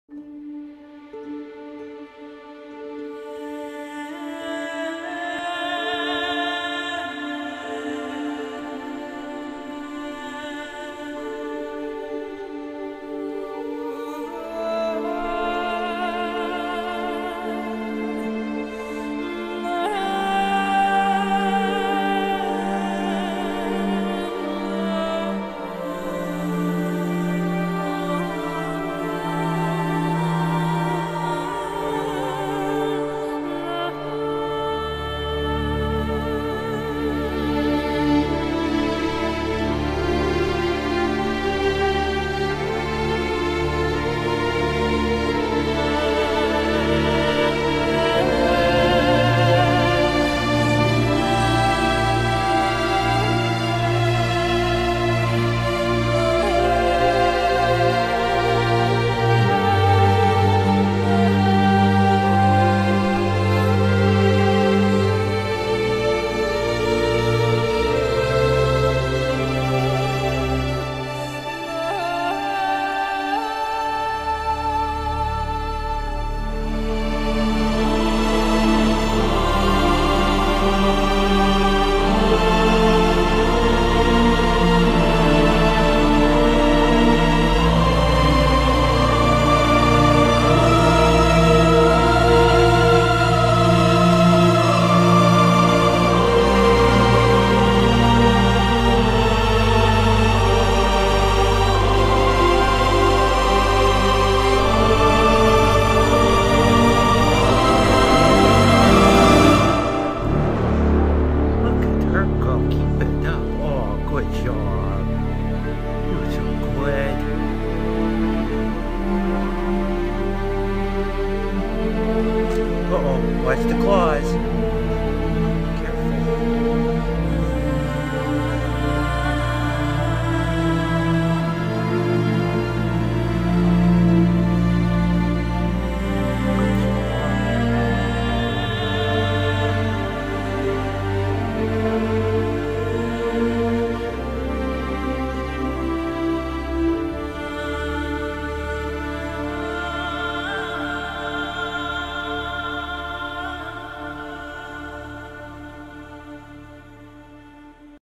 Catnip Painting 2
The kitty decided she wanted to paint again as the catnip  got her creative juices flowing.   She did green, pink, purple  and glow in the dark.  I think this was one of her best pieces yet.